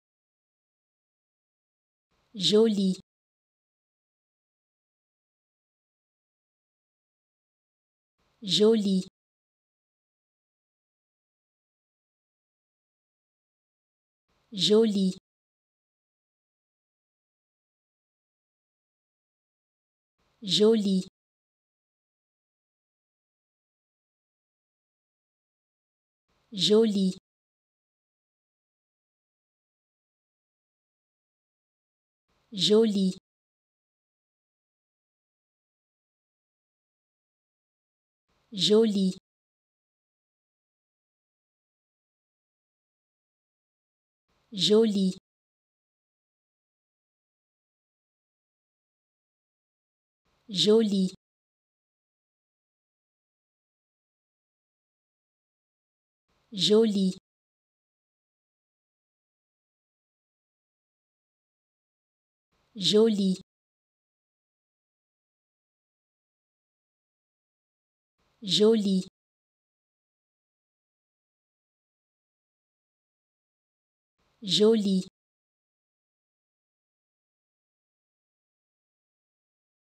How to pronounce ''Joli'' in French
Learn how to say and properly pronounce ''Joli'' in French with this free pronunciation tutorial. Apprenez à prononcer des mots en français grâce à nos tutoriels.